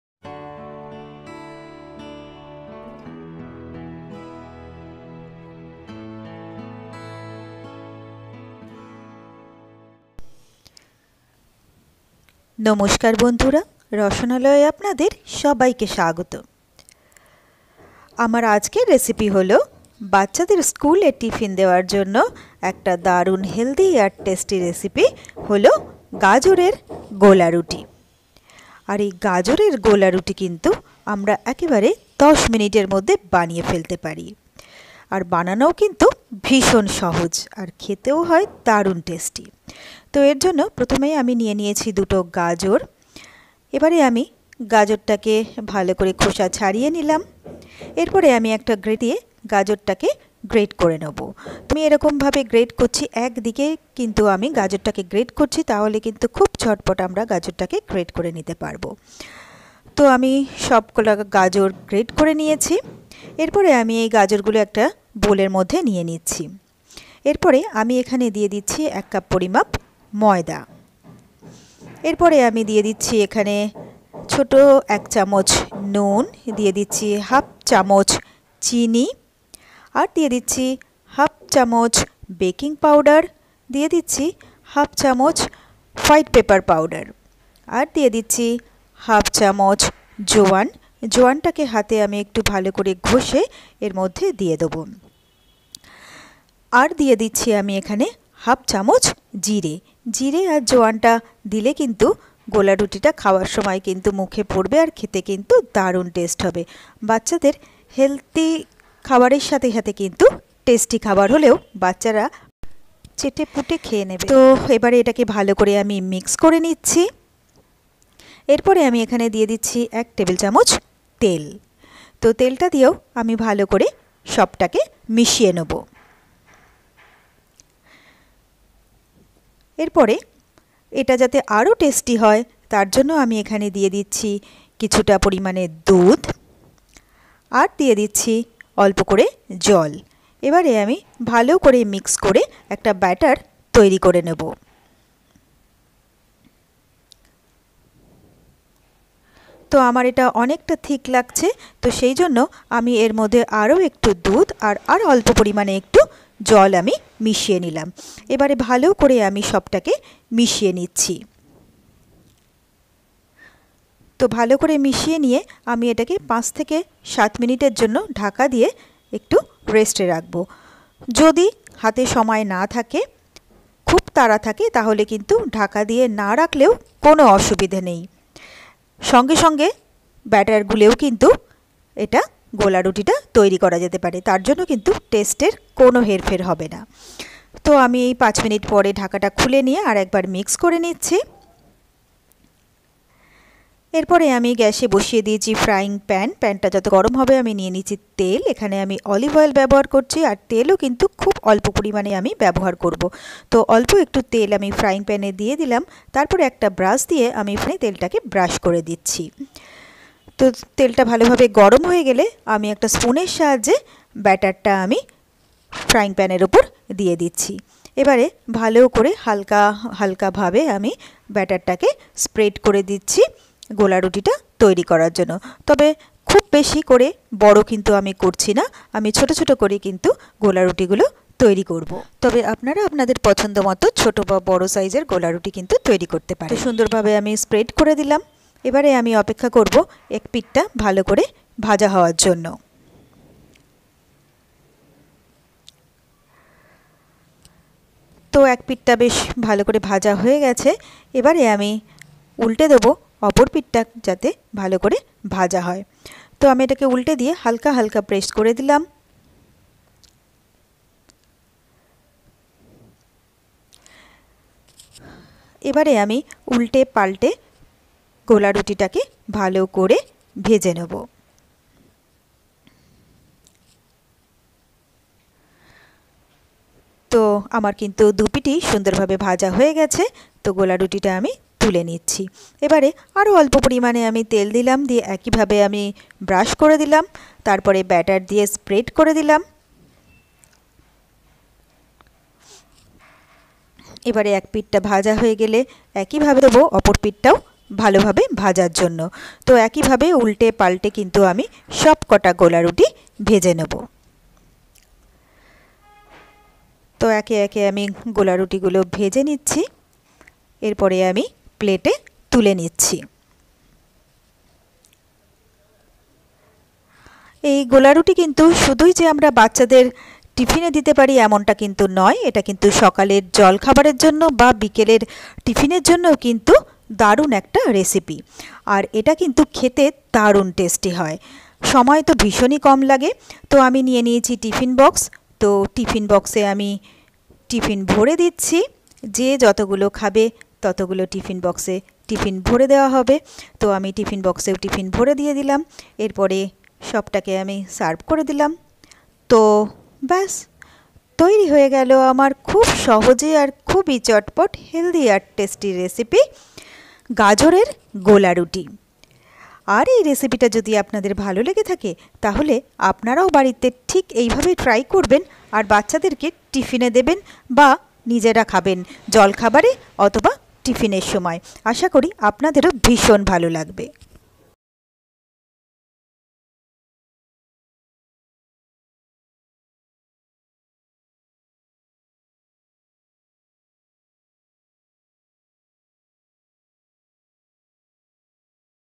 Carrot Pancake | গাজরের গোলারুটি বাচ্চাদের স্কুলের টিফিনের জন্য দারুণ সুস্বাদু ও স্বাস্থ্যকর রেসিপি
A narration on preparation of home made Carrot Pancake. The unique style of this simple recipe gives you a delicious tasty & healthy dish, perfect for kids lunch box, breakfast or tiffin. Go ahead and check it out.

Ingredients: Carrot; Milk; Salt; Sugar; White Pepper powder; Baking powder; Ajwaine; Cumin Seeds; Olive Oil.